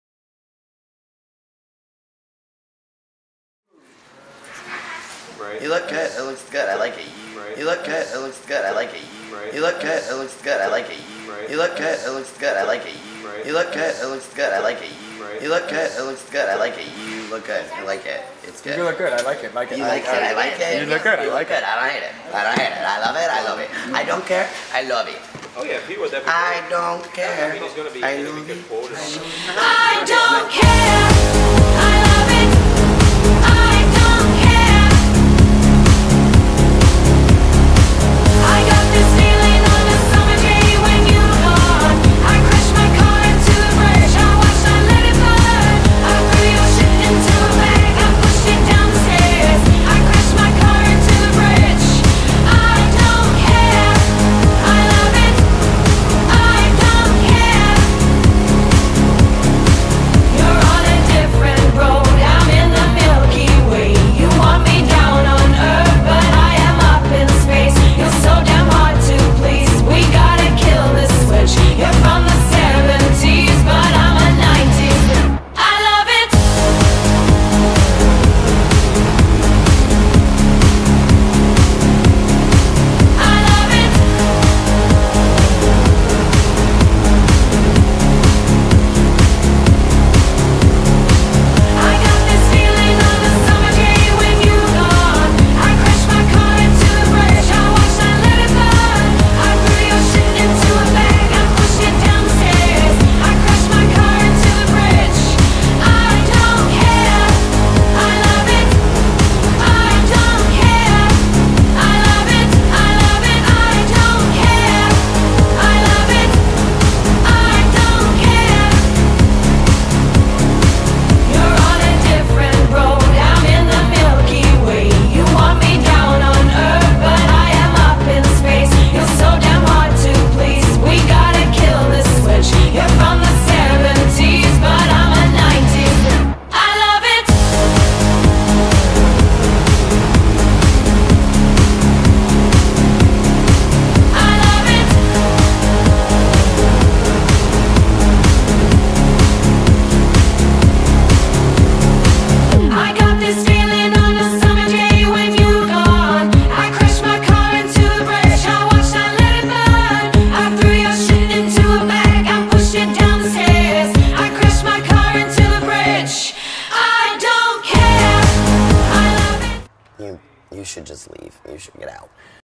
MOTH Poetic Circus in Action
A few outtakes from our video shoot rehearsal in San Antonio, January 2014.